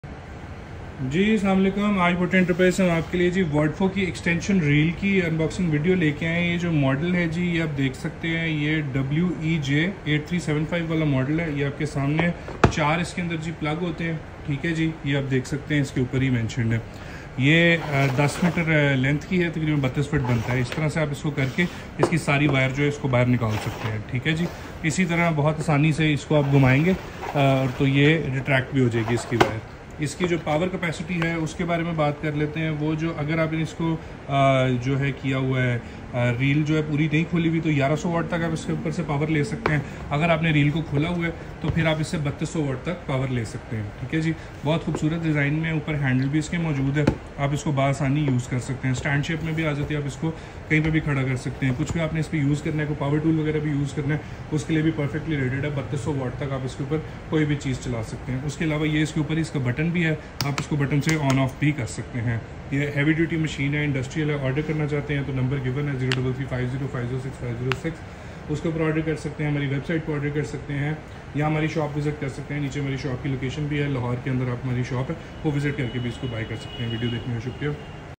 WADFOW Extension Reel WEJ8375 unboxing by Buttar Enterprises 03350506506 | Lahore Pakistan
WADFOW Extension Reel WEJ8375

Earthed contact sockets:
4pcs
Reel material: plastic
Cord: H05VV-F 3G1.5mm²
Cord length: 10m

ButtarEnterprises.com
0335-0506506
Main GT road, near Attock Petrol Pump, Salamatpura Container Market Lahore